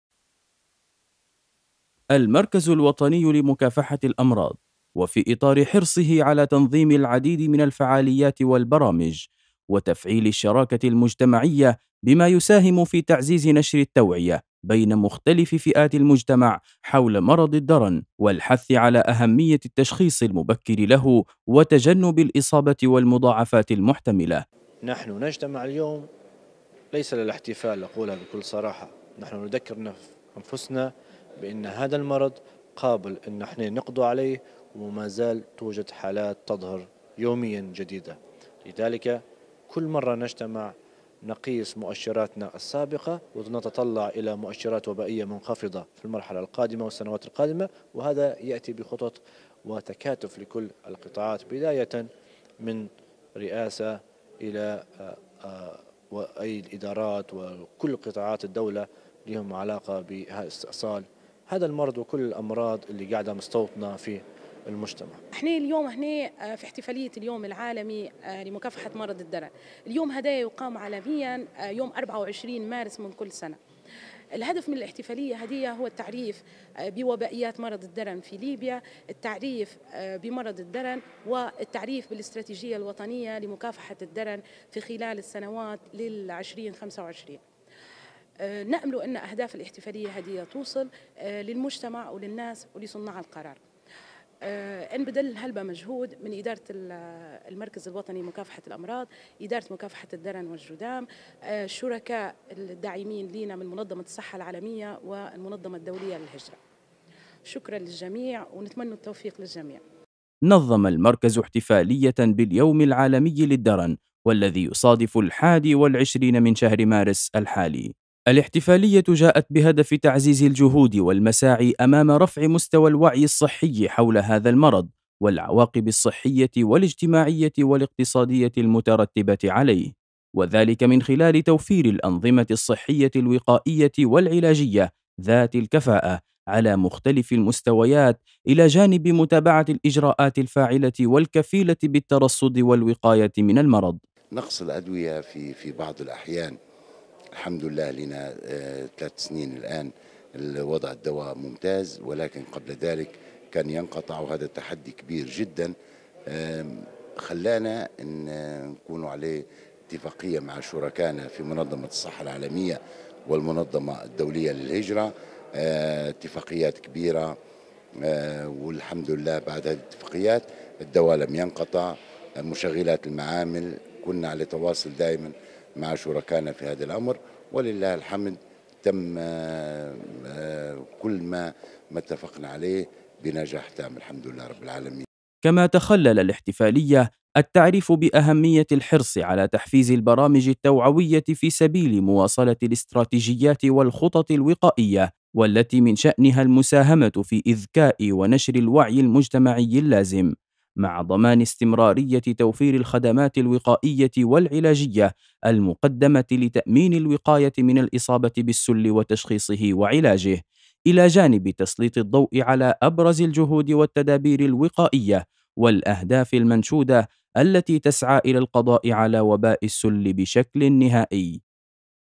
نظَّم المركزُ الوطني لمكافحة الأمراض بطرابلس احتفاليةً بمناسبة اليوم العالمي للدرن
تقرير - بلديات | نظَّم المركزُ الوطني لمكافحة الأمراض بطرابلس احتفاليةً بمناسبة اليوم العالمي للدرن، الذي يصادف الواحد والعشرين من شهر مارس الحالي، بهدف تعزيز الجهود والمساعي أمام رفع مستوي الوعي الصحي حول هذا المرض، والعواقبِ الصحية والاجتماعية والاقتصاديةِ المترتِبةِ عليه | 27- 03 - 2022

تشهد أسعار الخبز منذ اندلاع الحرب الروسية الأوكرانية ارتفاعا غير مسبوق مع تعالي أصوات الاتهام للتجار باستغلال الأزمة .. المزيد في سياق التقرير التالي

ــــــــــــــــــــــــــــــــــــــــــــــــــــــــــــــــــــــــــ
ــــــــــــــــــــــــــــــــــــــــــــــــــــــــــــــــــــــــــ
يمكنكم تحميل تطبيقنا TANASUH TV على الهواتف الذكية: